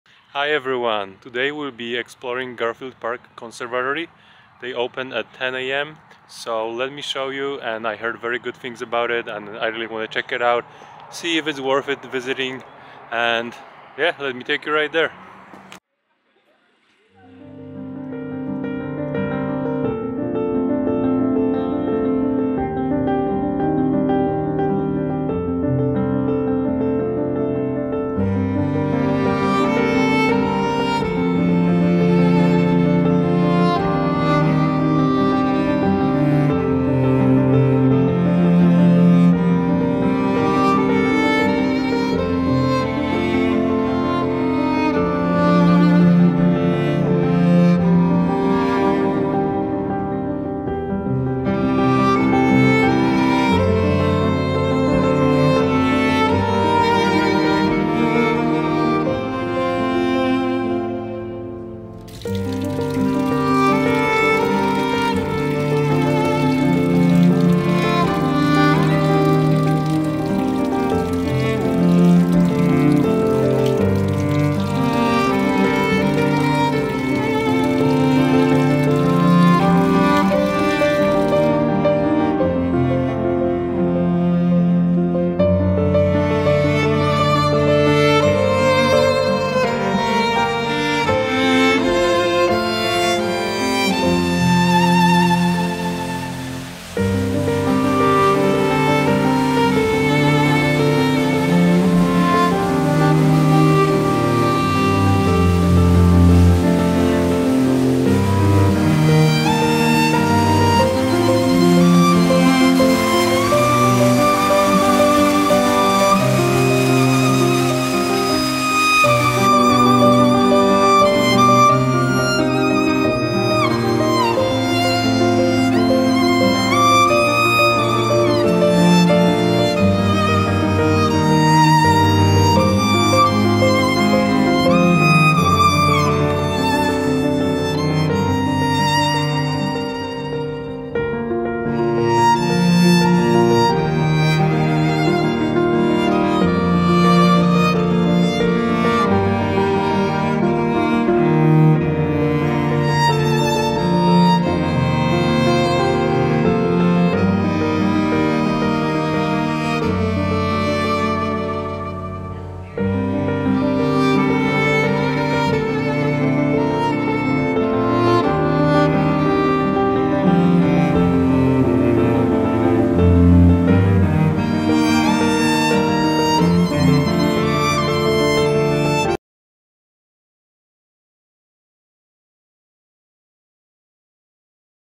Beauty of Garfield Park Conservatory in Chicago!
It is FREE!

Garfield Park Conservatory is a botanical garden and greenhouse located in Chicago, Illinois, USA. It spans 12 acres and features a wide variety of indoor and outdoor plant displays, including tropical and desert plants, ferns, cacti, orchids, and palm trees.

The conservatory was designed by renowned landscape architect Jens Jensen and opened to the public in 1908. It is one of the largest conservatories in the United States and is listed on the National Register of Historic Places.

Visitors to the Garfield Park Conservatory can explore eight indoor display gardens, each with its own unique theme and plant collection, as well as several outdoor gardens and sculptures. The conservatory also offers educational programs and events, including guided tours, workshops, and classes for children and adults.

In addition to its botanical displays, the Garfield Park Conservatory is committed to environmental sustainability and operates a number of green initiatives, including rainwater harvesting, composting, and solar power.